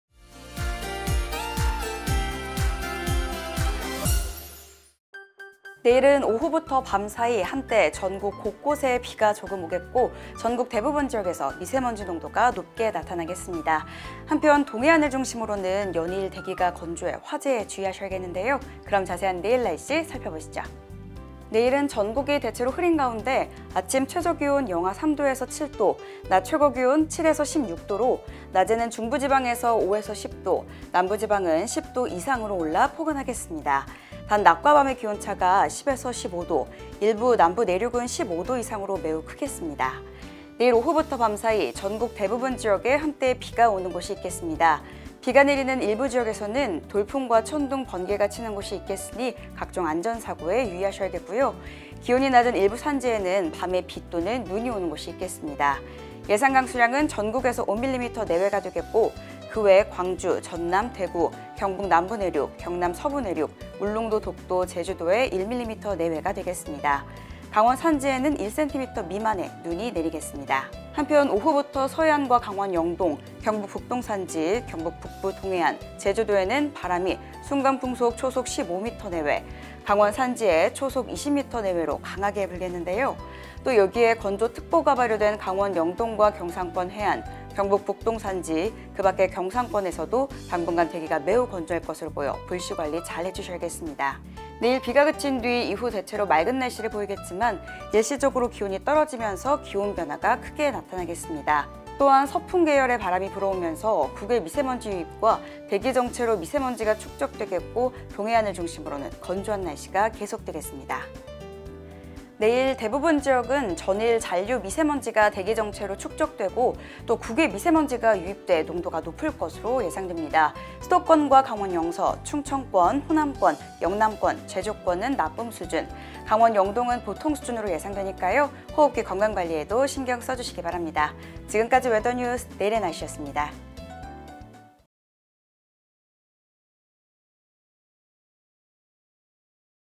[웨더뉴스] 내일의 날씨 (12월 06일)
내일은 오후부터 밤사이 한때 전국 곳곳에 비가 조금 오겠고,
전국 대부분 지역에서 미세먼지 농도가 높게 나타나겠습니다.

한편, 동해안을 중심으로는 연일 대기가 건조해 화재에 주의하셔야겠습니다.

오늘의 캐스터 : 김지수

더욱 자세한 날씨가 궁금하다면 웨더뉴스 날씨 정보 보러 가기